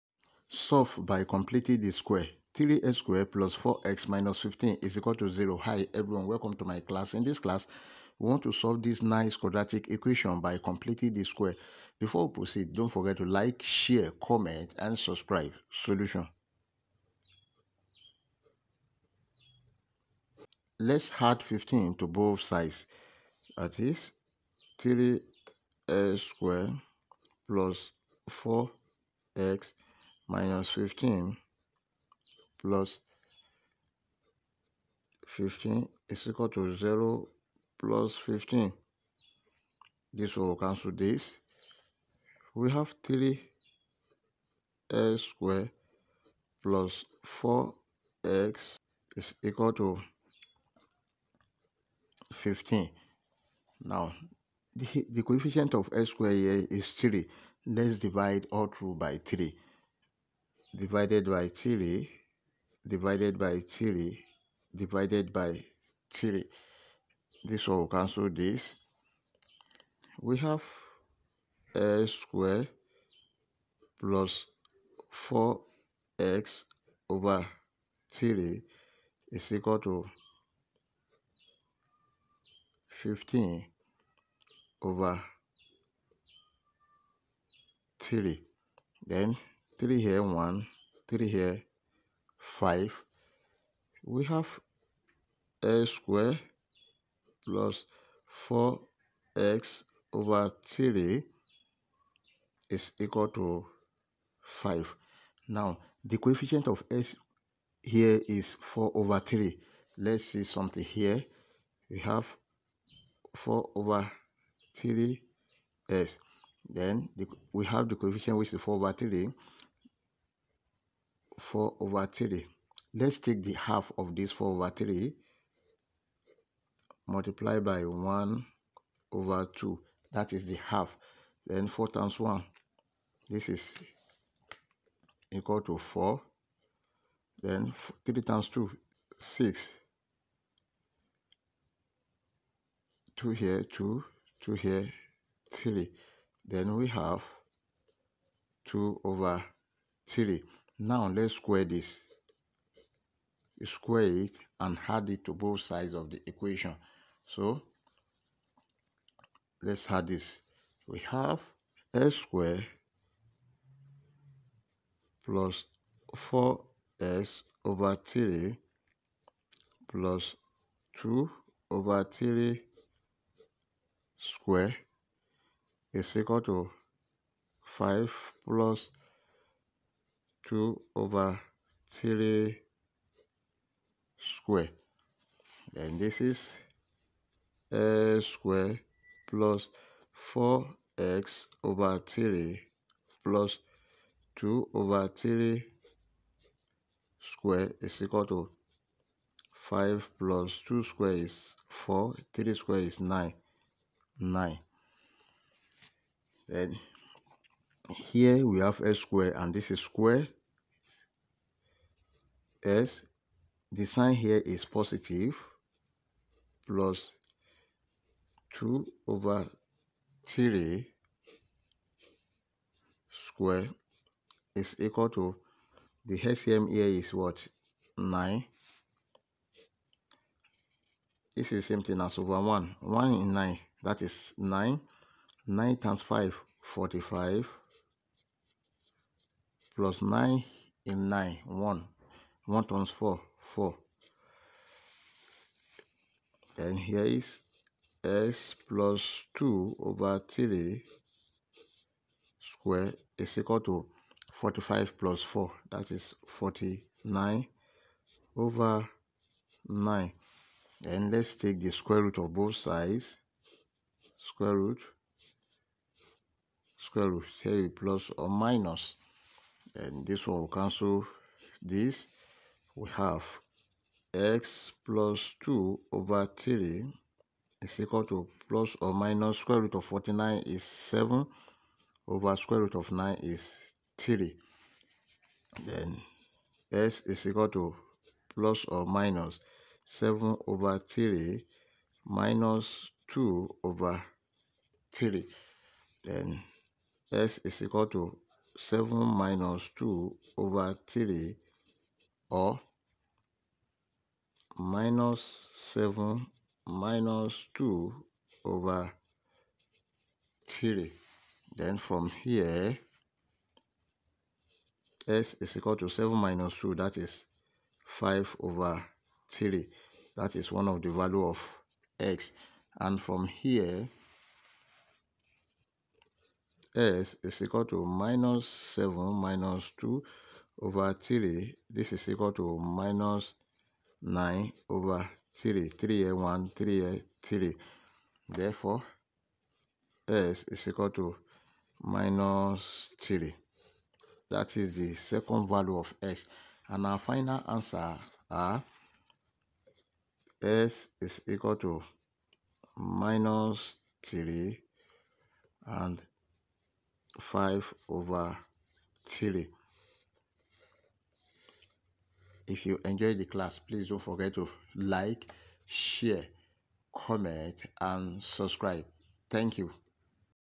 Nice Math Problem 3x^2+4x-15=0 | Solve By completing The Square | Step By Step Guide...
In This video, you will learn how to the given nice quadratic equation by completing the Square very fast. Here are step-by-step technique.